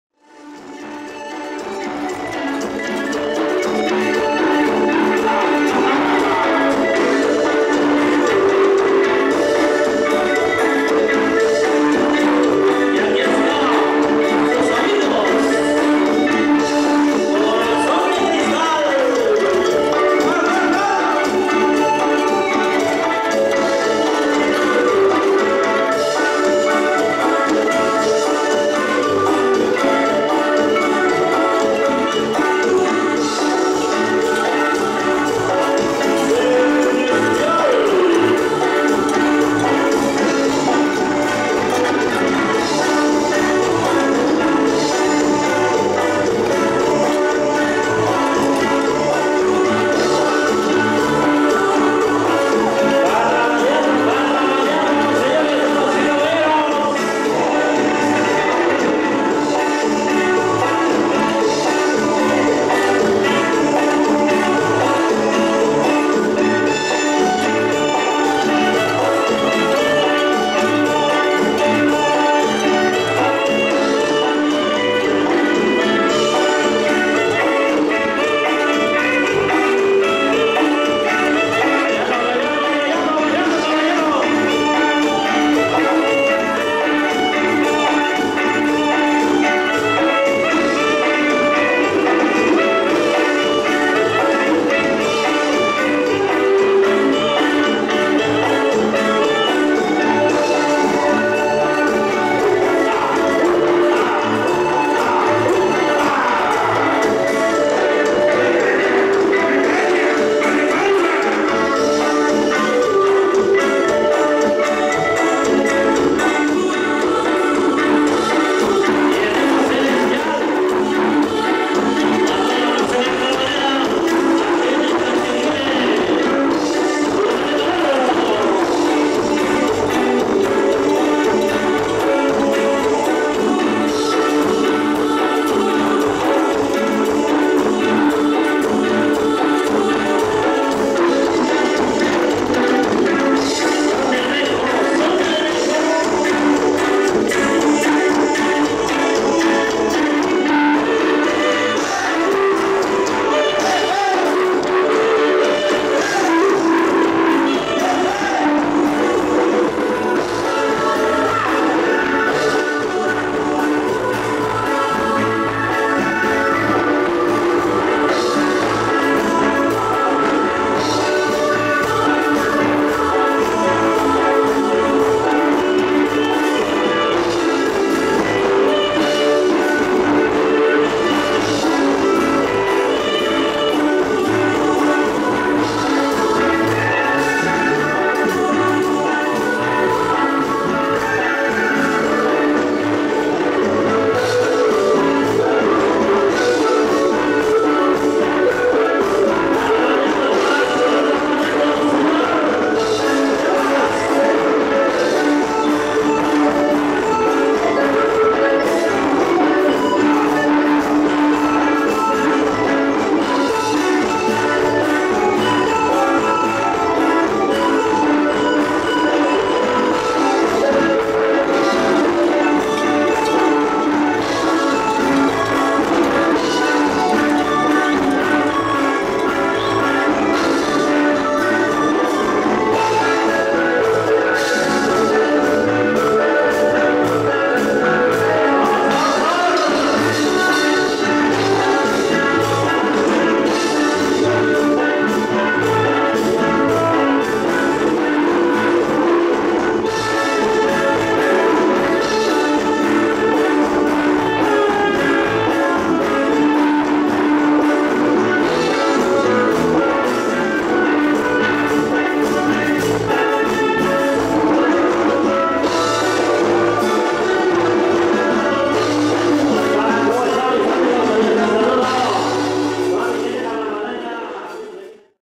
Cierre de Carnaval 2020 Tlachiquile ,Ixh. de Madero; Con el Grupo Corazón de Cristal
Cierre de Carnaval en Tlachiquile , Ixhuatlán de Madero con el Grupo Corazón de Cristal ; De Tenextitla ,Chicontepec ; Ver.